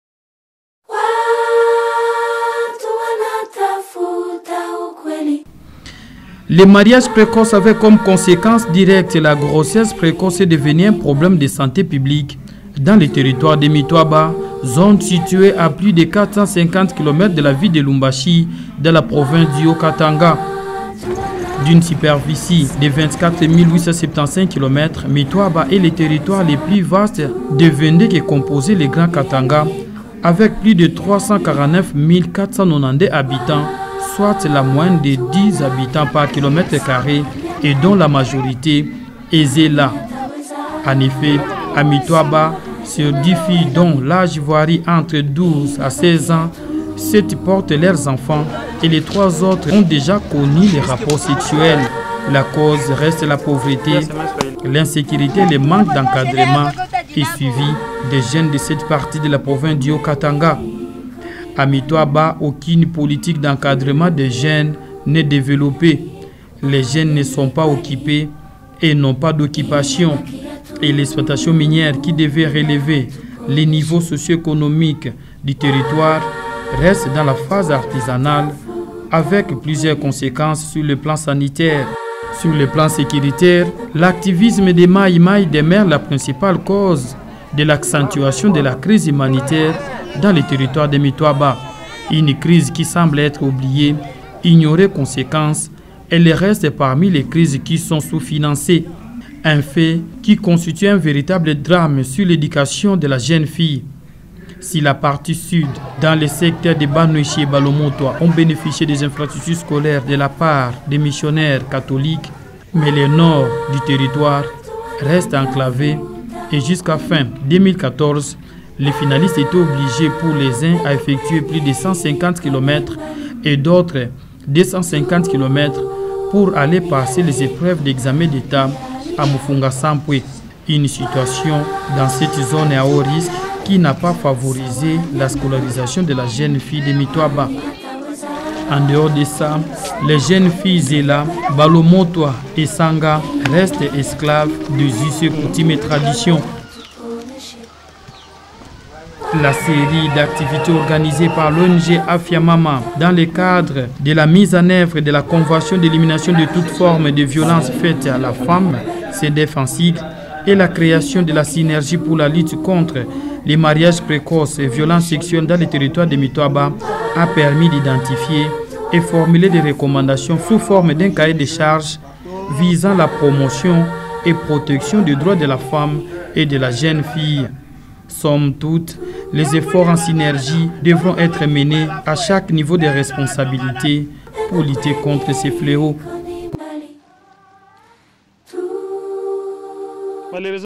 MITWABA MARIAGE PRECOCE- AFIA MAMA
Les mariages précoces avec comme conséquence directe les grosses précoces est devenu un problème de santé public dans le territoire de Mitwaba